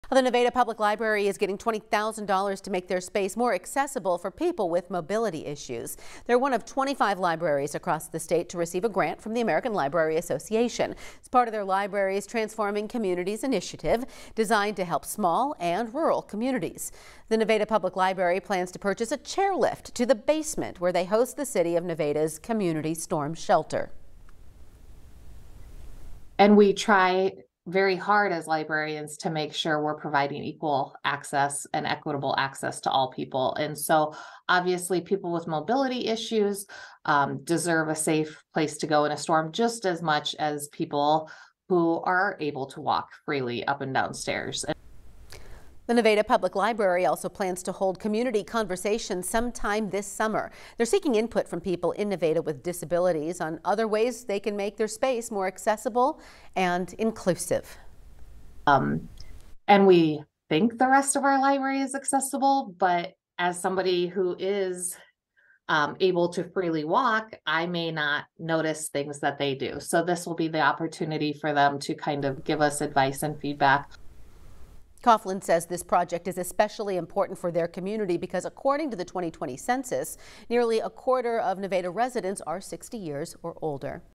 Nevada Public Library director discusses importance of accessibility
The Nevada Public Library is one of 25 libraries from across the state to receive a grant from the American Library Association.